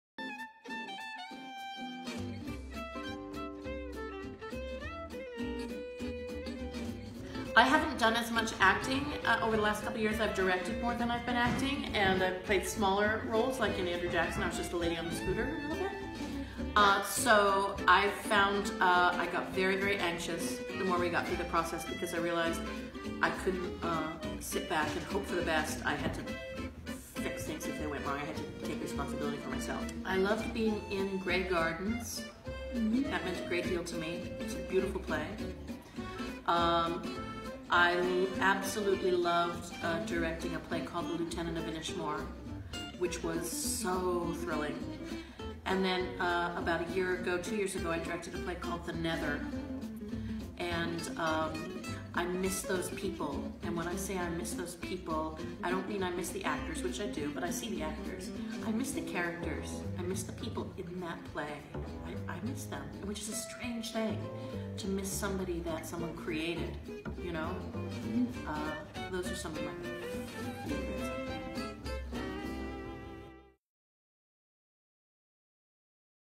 Mad Horse Minute, Episode 3
A quick interview with Christine Louise Marshall who plays Cynthia in "Deer" by Aaron Mark and is a Mad Horse Theatre Company member